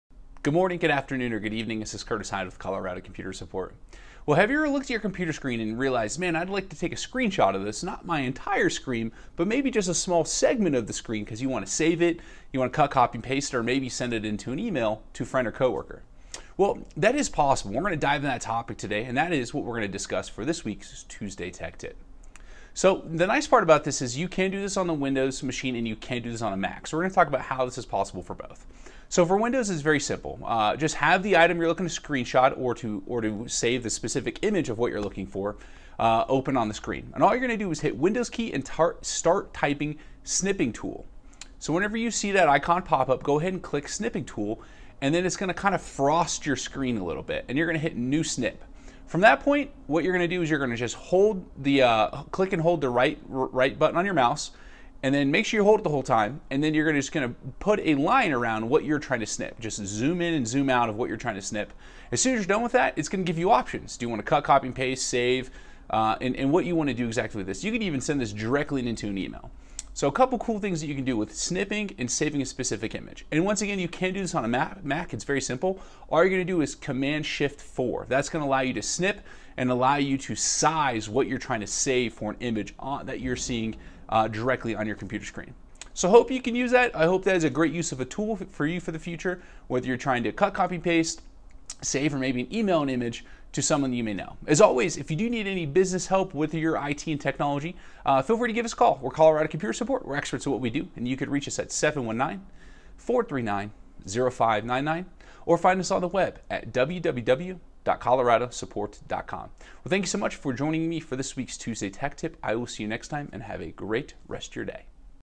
How Can You Take A Screen Shot for Windows and Mac | Colorado Computer Support
How can you take a screen shot on Windows or Mac? Curtis Hyde from Colorado Computer Support shows you how.
For more information about Colorado Computer Support and our services,